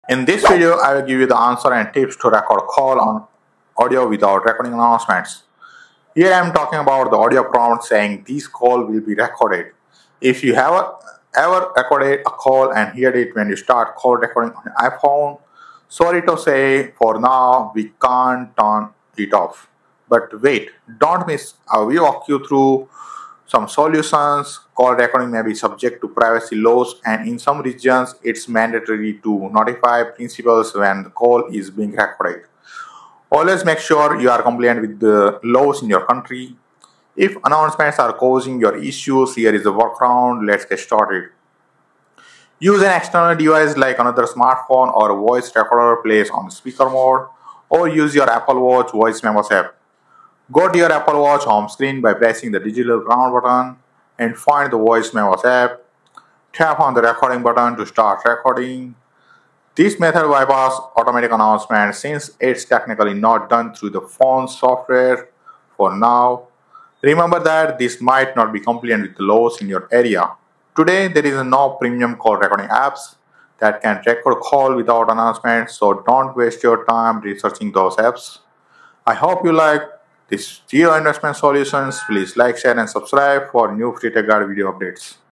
Can You Do Call Recording in iPhone Without Announcement? (2 Ways) - Here's Why
Record iPhone call without Audio Prompt Recording Announcements on iPhone Calls! Here's Two Tricks that work you can record call! on iPhone 16 (Pro Max), iPhone 15 (Pro Max), iPhone 14 (Pro Max), iPhone 13 (Pro Max), iPhone 12 (Pro Max), iPhone 11 (Pro Max). iPhone users asking for Can I Stop "This Call Will Be Recorded" Announcements on my iPhone? Fix  Why!

00:00 intro
00:15 Here's Answer
00:33 Record Call in 2 Ways